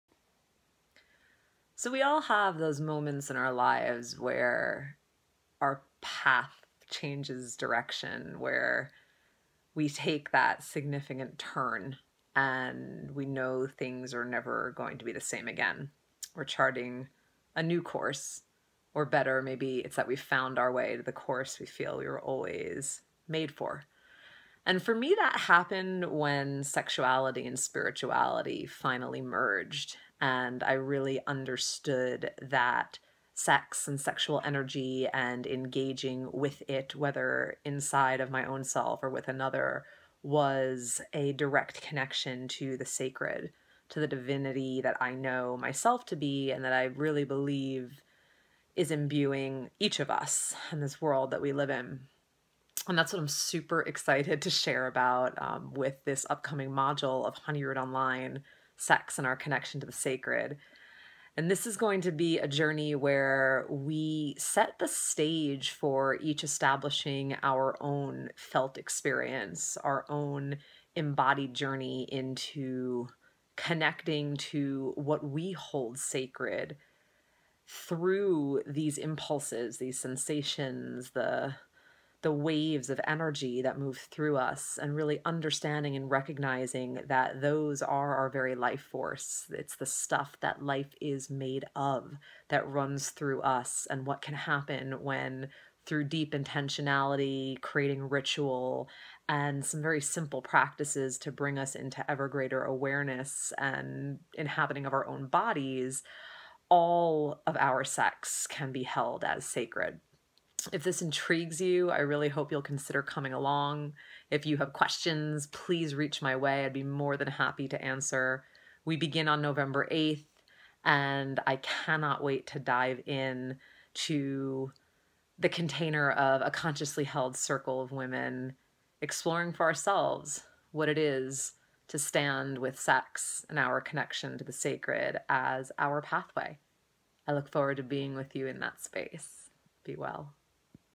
Sex and Our Connection to the Sacred
Hear from Darshana Avila, who will be guiding this journey as part of HoneyRoot Online, as she shares what this work has meant for her personally and what you can expect from your own exploration. and we begin November 8, 2016.